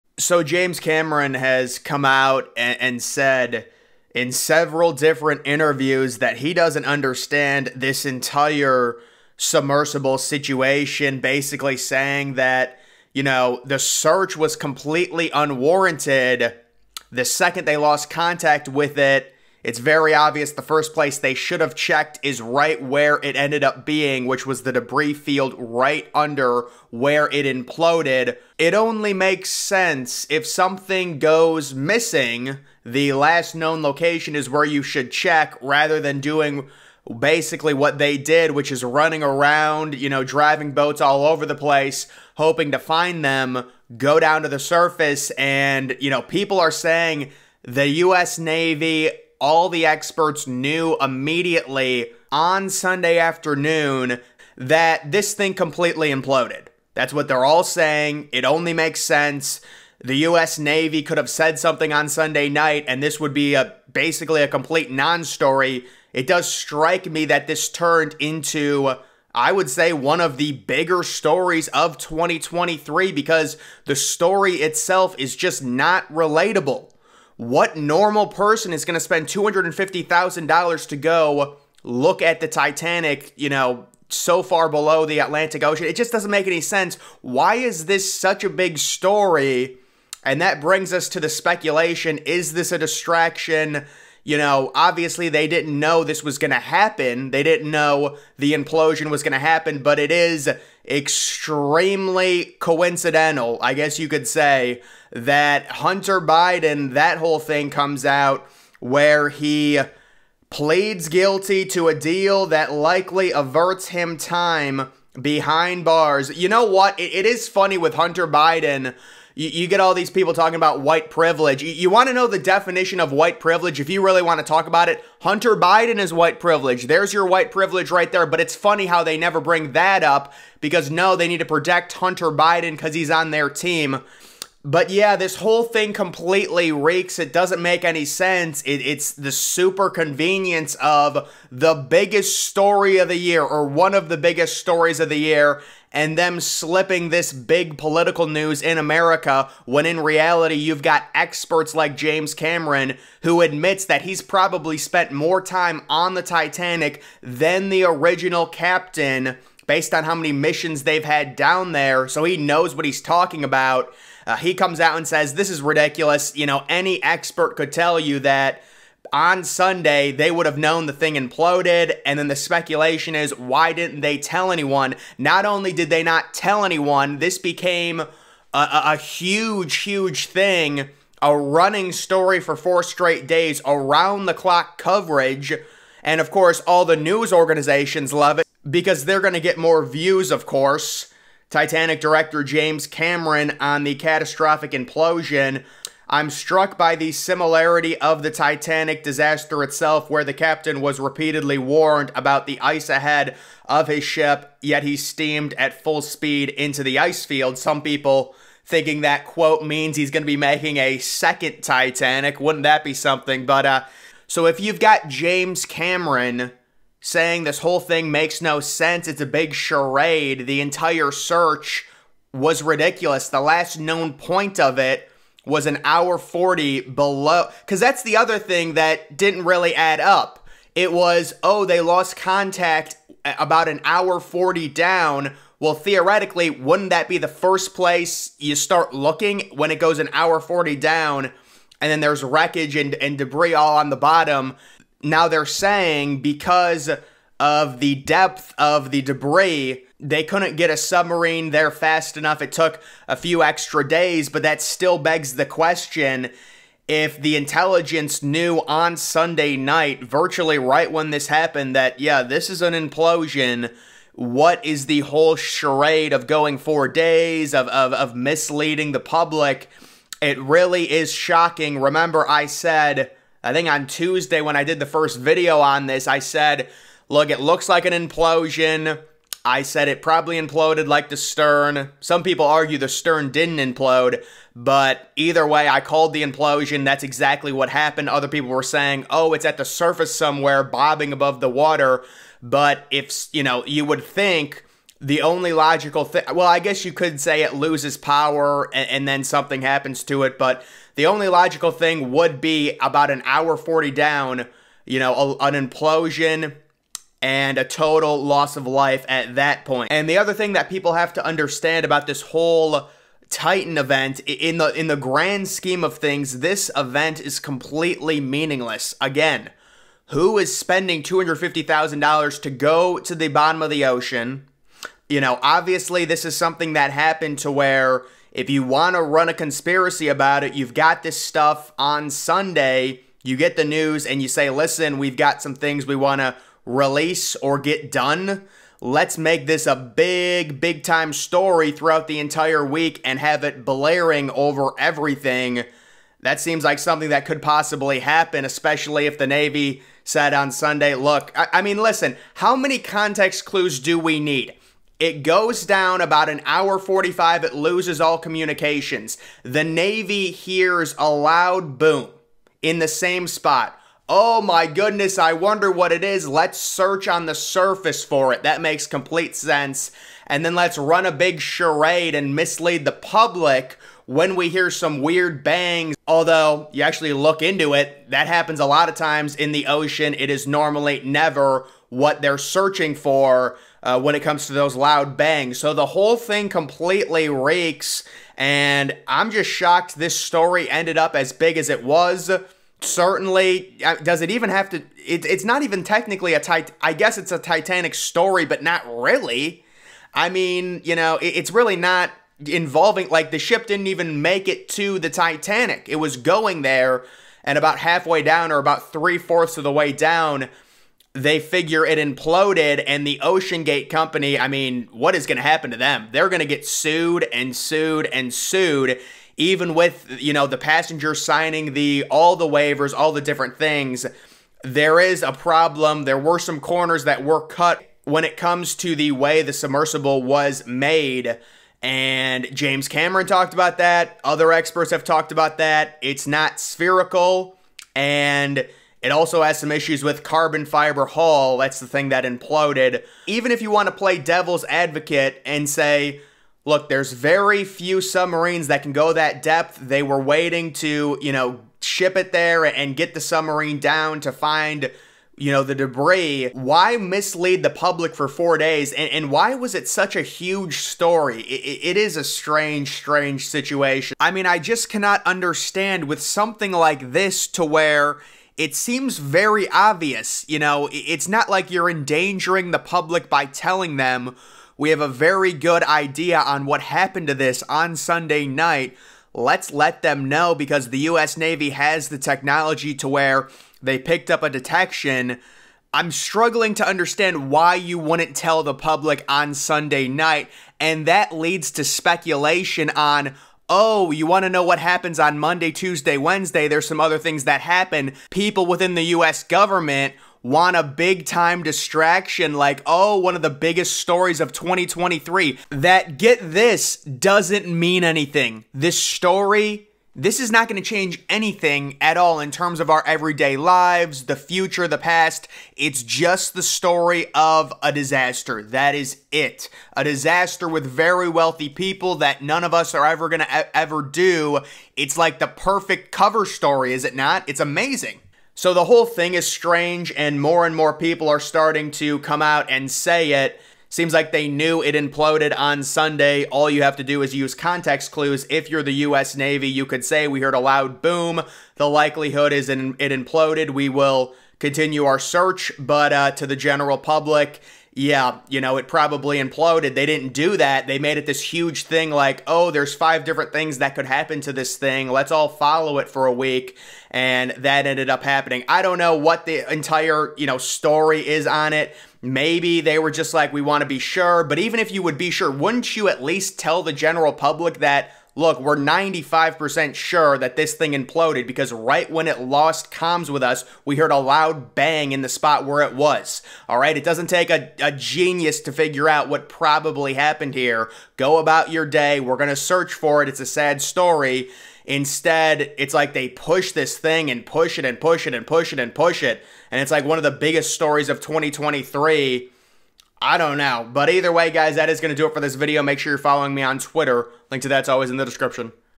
James Cameron *SLAMS* Titanic Titan search "waste of time"
Discussing the Titan missing submarine search and the issues with it